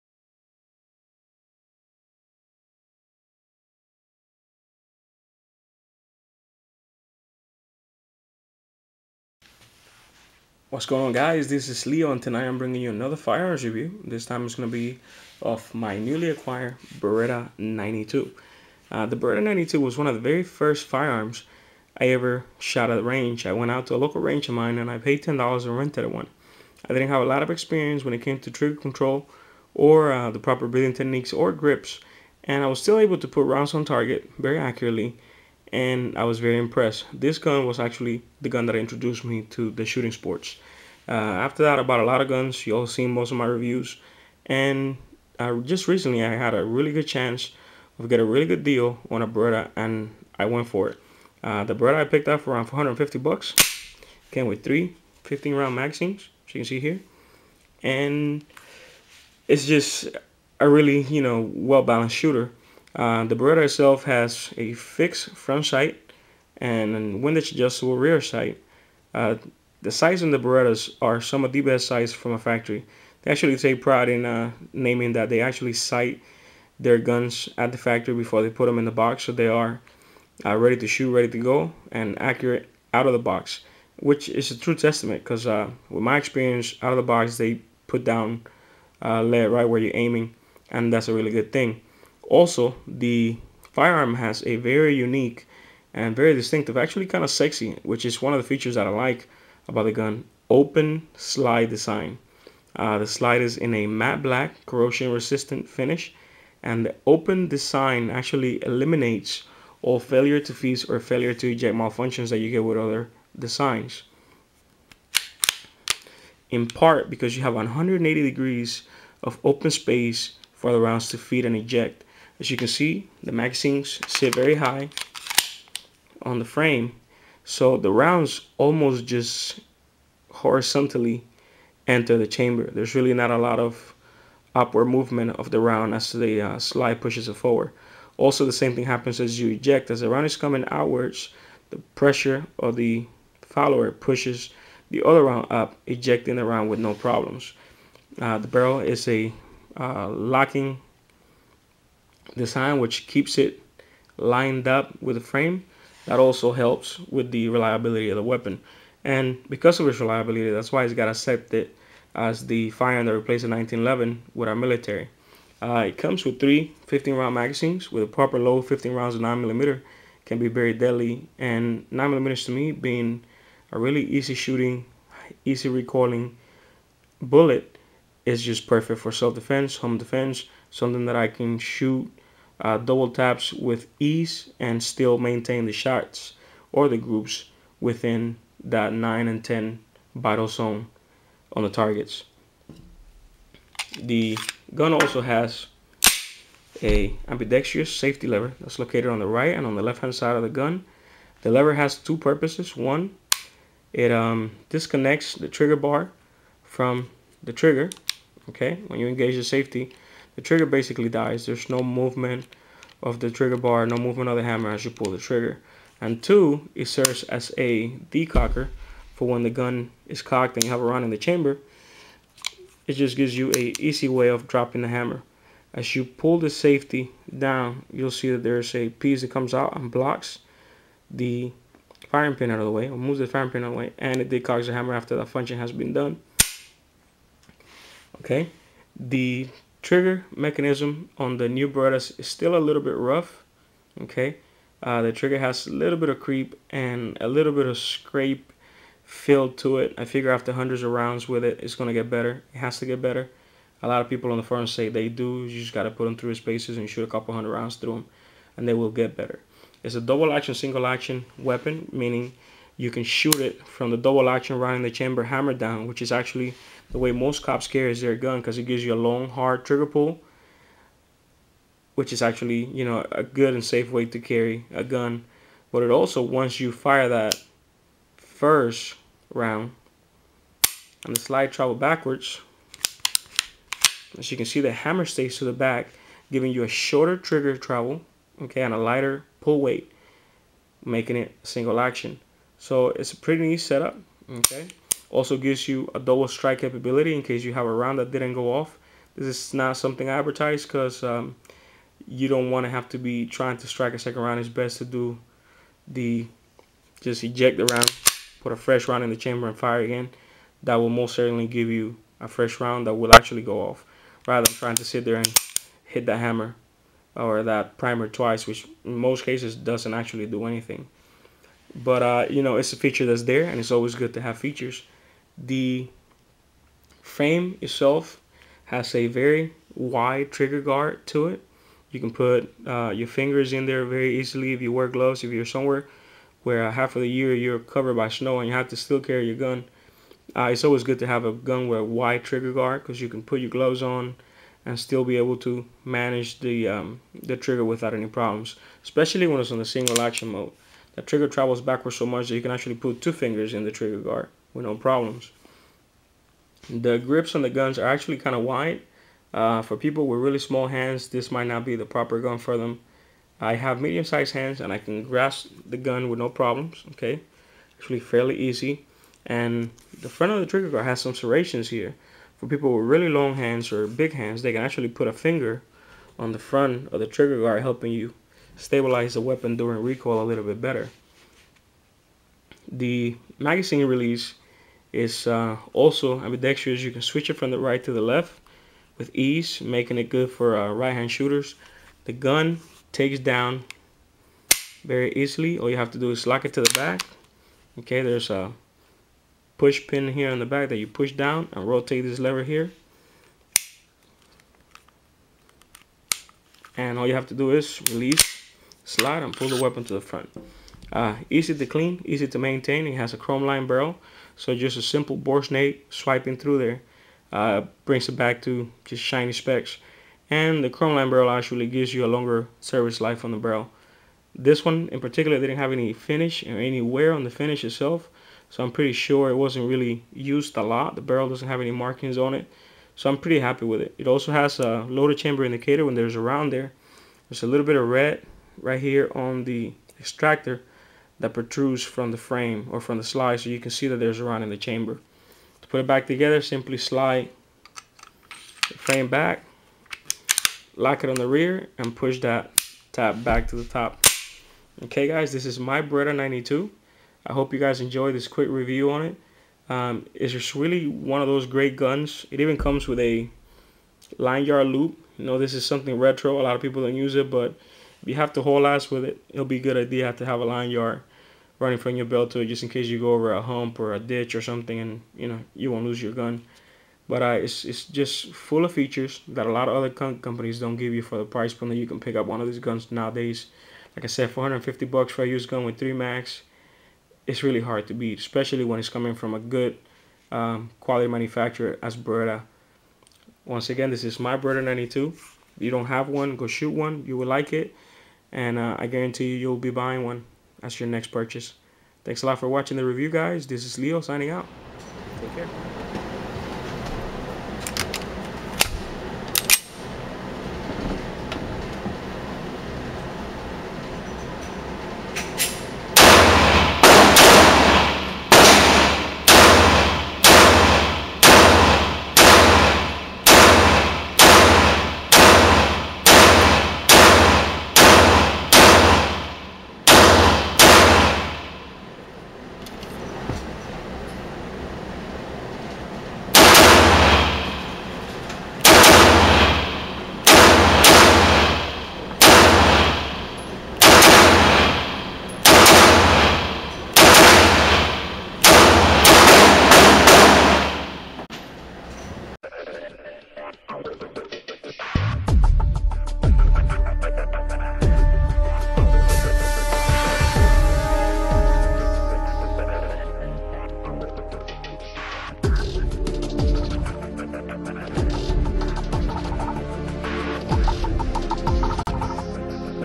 Beretta 92FS Review
ATT: I paid 450 for the gun Not 150 Just clearing the air.

This is going to be my review on the Beretta 92FS. The 92FS was the gun that got me introduce into shooting sports. I had the pleasure to use a Beretta 92FS as the very first gun I ever fired and from that excellent first experience is that I now Understand and Support the Second amendment.